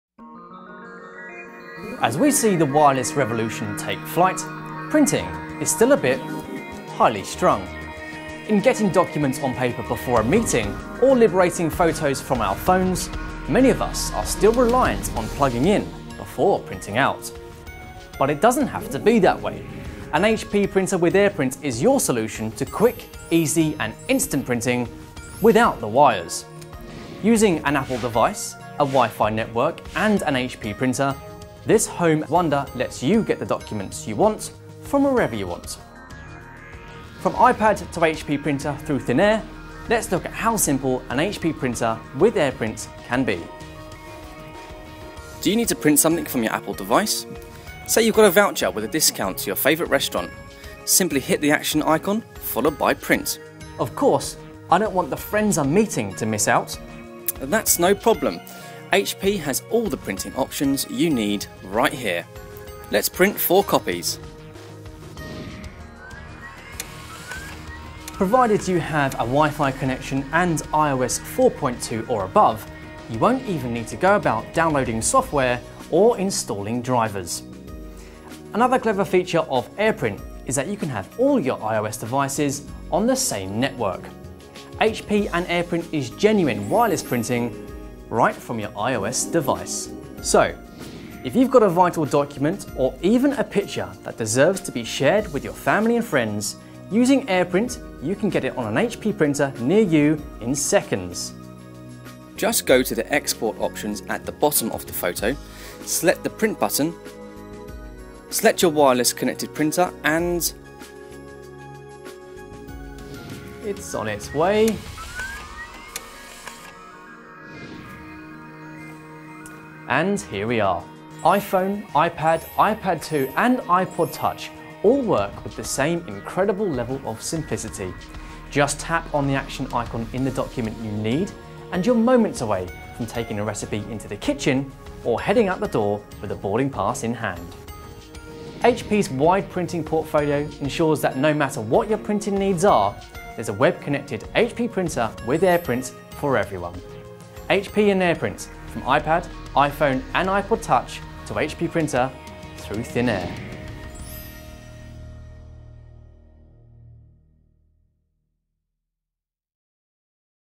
HP and AirPrint - This is How it Works
An HP Printer with AirPrint is your solution to quick, easy and instant printing without wires from your iOS devices. Simply print wirelessly from your iPad®, iPhone®, and iPod Touch® to HP web-connected printers. Using an Apple device, a wi-fi network and an HP Printer (featured: HP ENVY 110 e-All-in-One), this home wonder lets you get the documents you want from wherever you want. From iPad to HP Printer through THIN AIR. See how easy it is to make it work.

Special thanks to Dennis Publishing for producing this video for HP